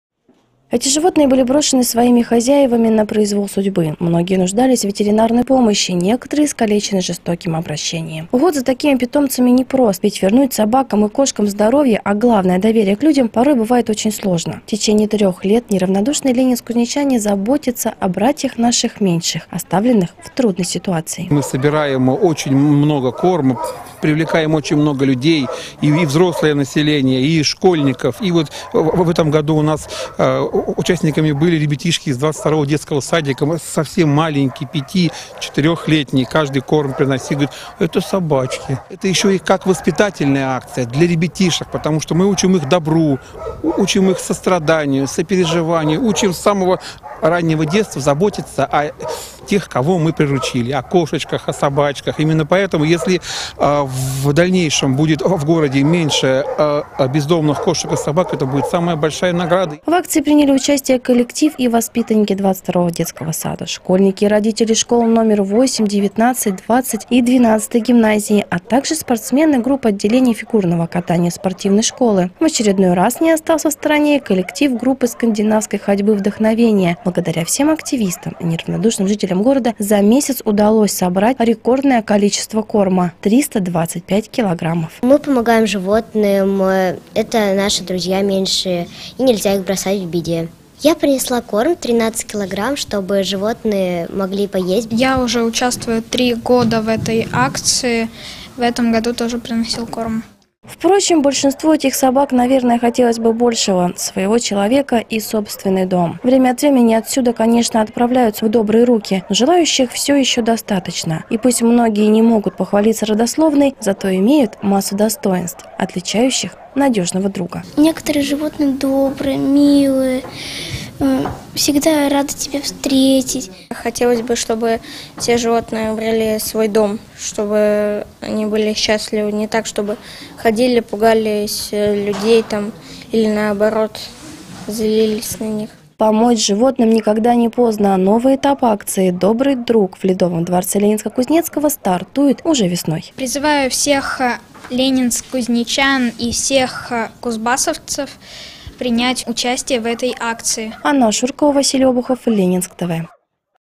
На базе Ледового дворца прошла благотворительная акция «Добрый друг»
На базе Ледового дворца прошла благотворительная акция по сбору корма для бездомных животных, находящихся в пунктах передержки на территории Ленинск-Кузнецкого - «Добрый друг». Ежегодно, акция проходит в рамках областного благотворительного проекта «Тёплый Кузбасс» при поддержке активистов Кемеровского регионального отделения Всероссийской политической партии «Единая Россия».